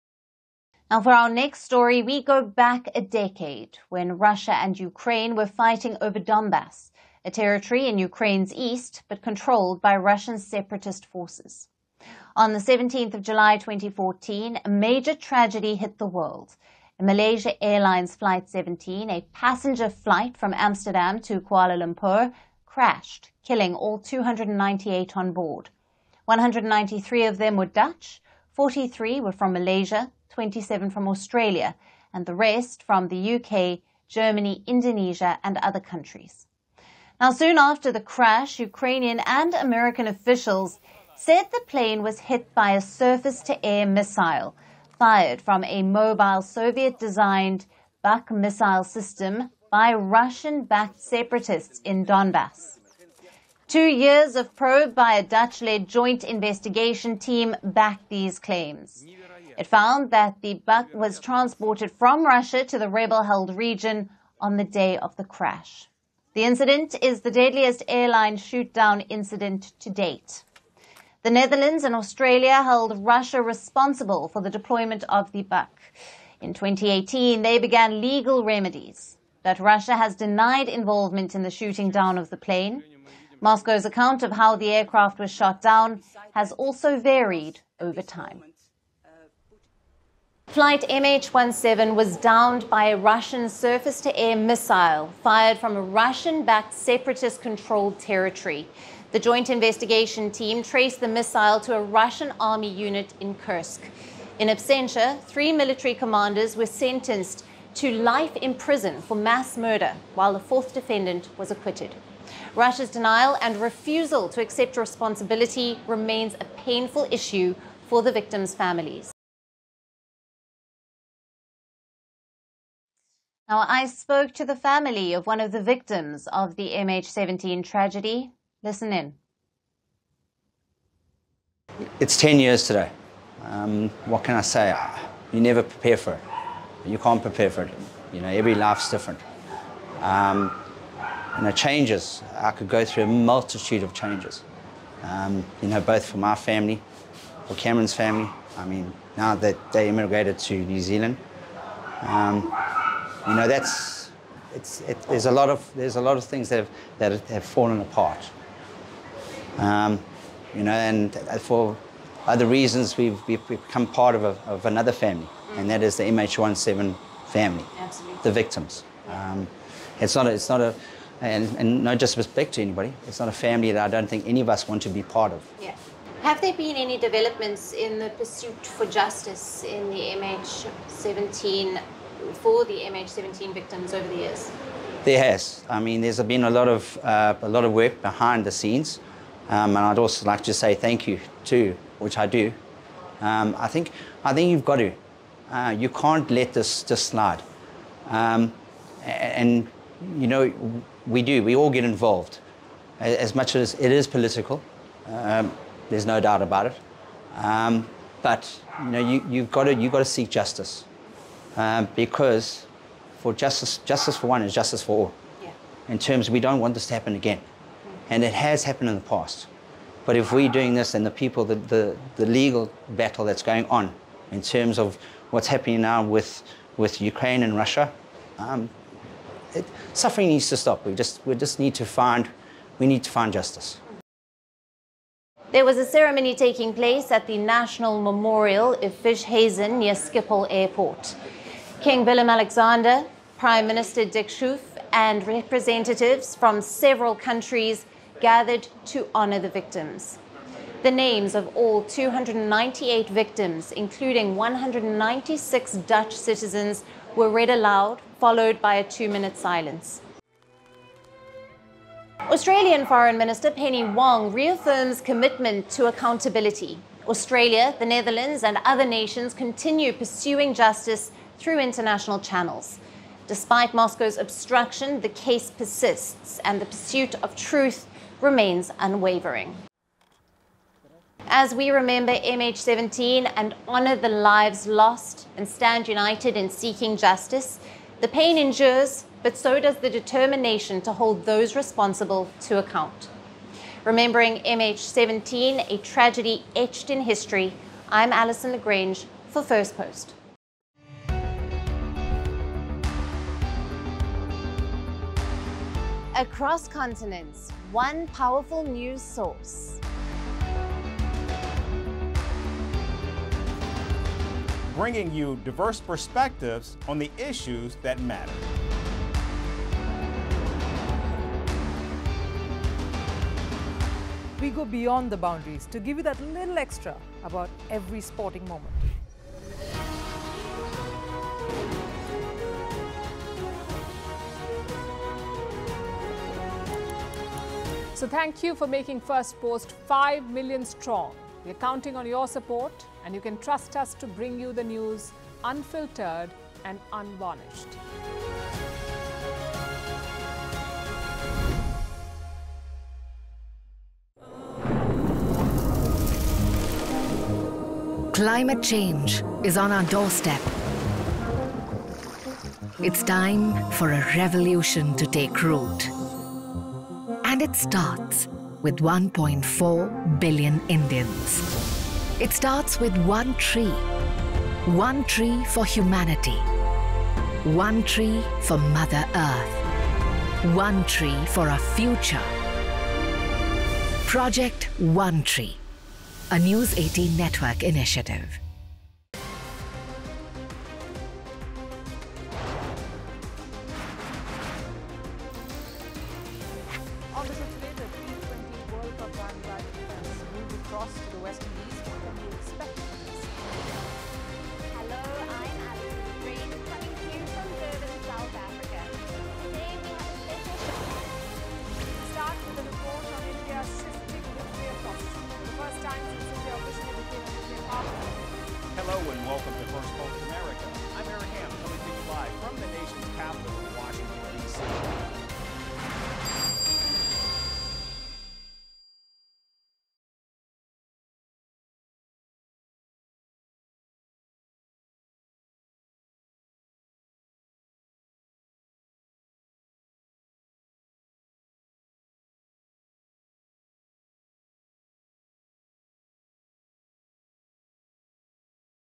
MH17 Crash: What Happened 10 Years Ago? Families Still Await Justice | Firstpost Africa
A decade ago, on 17th July 2014, a massive tragedy shook the world. A Malaysia Airlines Flight 17, carrying 298 people on board from Amsterdam to Kuala Lumpur crashed with no survivors. 193 of those on board were Dutch, 43 were from Malaysia, 27 were from Australia and the rest from UK, Germany and other countries. The flight was hit by a surface-to-air missile fired by Russia-backed separatists in the Donbas region. The Netherlands commemorated the 10th anniversary of the airline disaster. 10 years on, the families of victims still await justice. Firstpost spoke to the family of one of the victims of the MH17 crash.